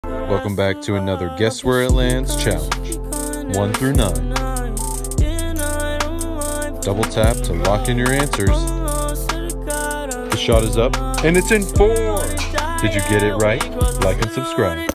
Can “YOU” Guess where it lands!? GOLF CHALLENGE!🎯💦
Chip Shot is here to bring the prestigious game of golf to everyone! By creating a competitive party game thats affordable, fast paced, and fun for the whole family!

Chip Shot has taken inspiration from multiple popular family favorites such as corn hole, and combined them into one high octane party game!

No more spending hundreds of dollars on a nice round of golf, just to sit and wait on every tee box for the group in front. Gather up to 4 players and face off in this highly competitive fast paced game!

Each set comes with:

x10 CHIP SHOT Cups | (6)Blue(3)Yellow(1)Red
x3 CHIP SHOT Golf Balls
x1 CHIP SHOT Golf Bag